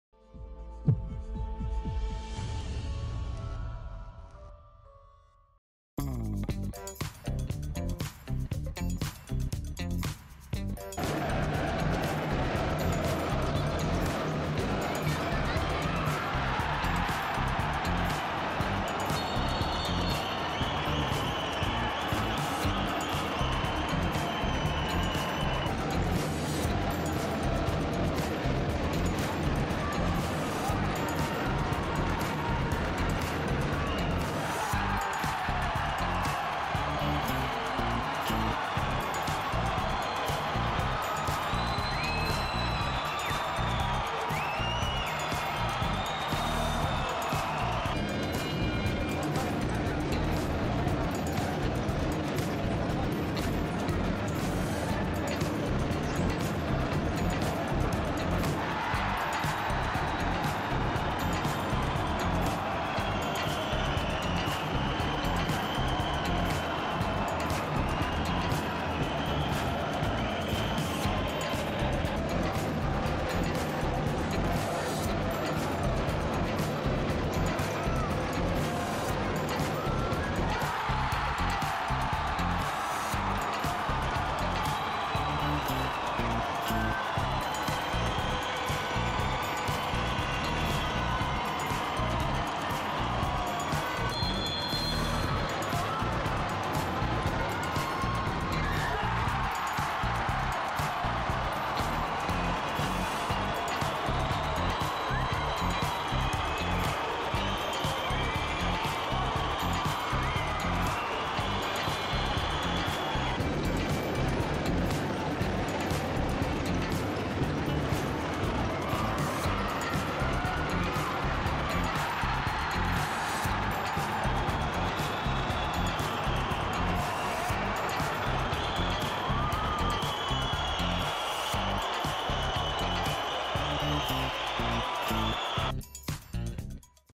LNF2018 - Magnus 3 x 3 Corinthians - Gols - 16ª Rodada
Gols:
Magnus - Sacon, Tiago e Rodrigo
Corinthians - Henrique (2) e Douglas Nunes